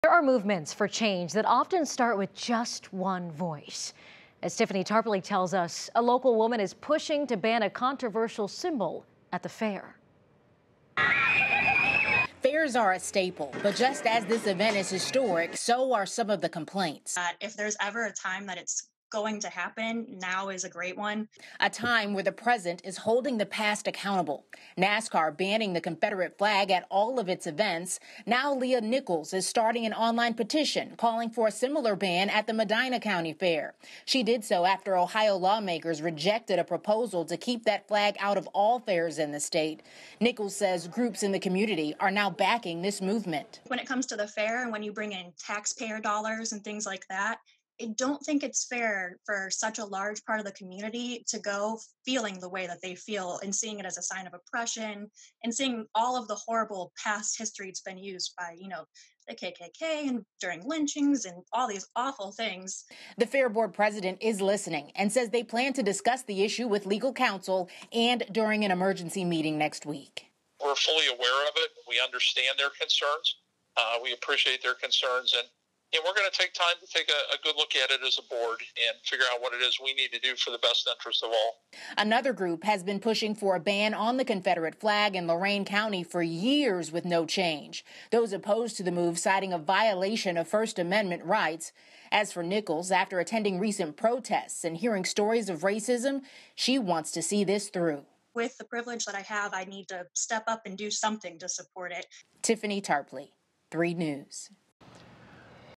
Local woman creates online petition to ban confederate flag at Medina County Fair
There are movements for change that often start with just one voice. As Tiffany Tarpley tells us, a local woman is pushing to ban a controversial symbol at the fair.